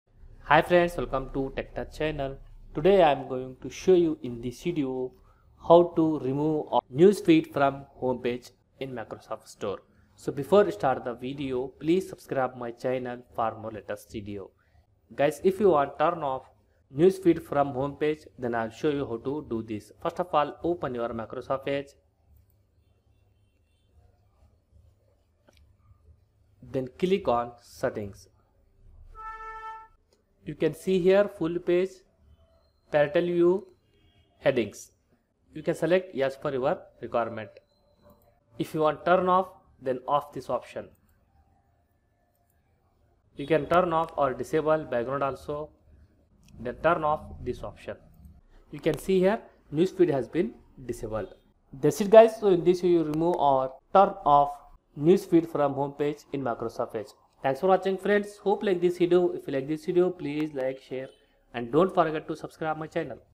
Turn Off Or Remove News Feed From Home Page on Microsoft Edge
Microsoft Edge Turn off News Feed in Microsoft Edge
How to turn off The News Feed on Microsoft Edge